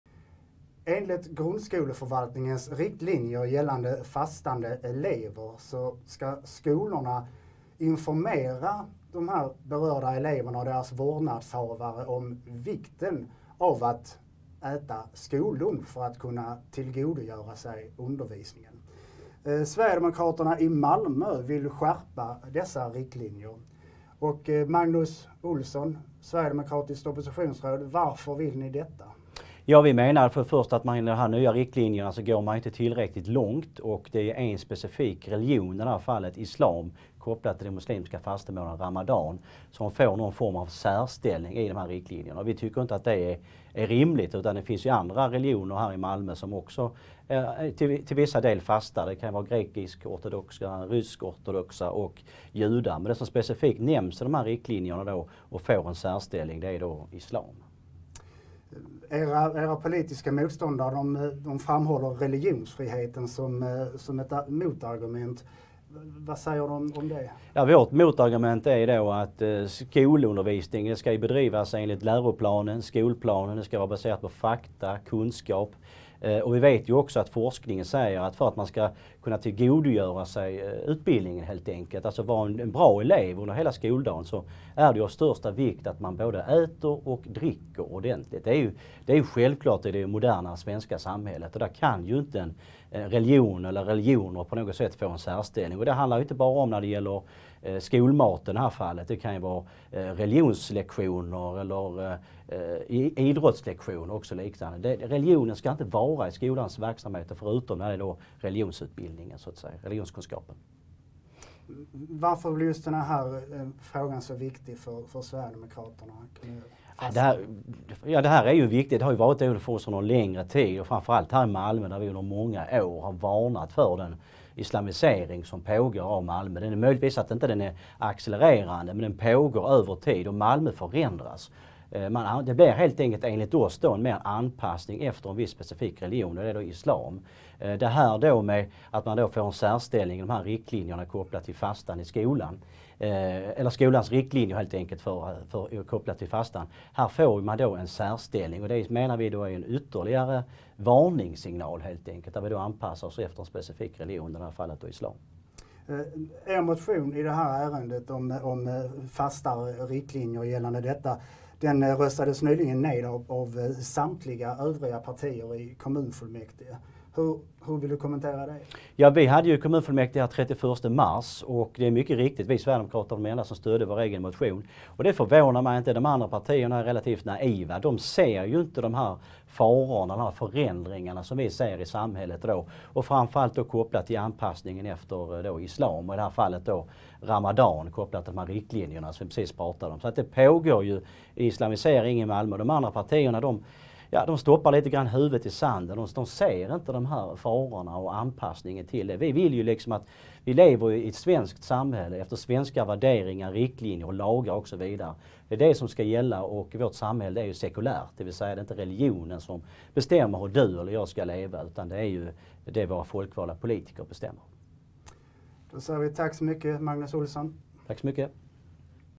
Den svenska skolan ska inte islamiseras! Intervju med oppositionsrådet Magnus Olsson (SD)
Barn ska inte tvingas fasta med religiösa förtecken och utbildningsväsendet ska inte på något sätt anpassas till islam. Undervisningen måste prioriteras i skolan och elever behöver äta för att orka med skoldagen.

När vi lyfte detta i Malmö kommunfullmäktige den 31 mars var vi det enda partiet som ville markera mot detta vansinne.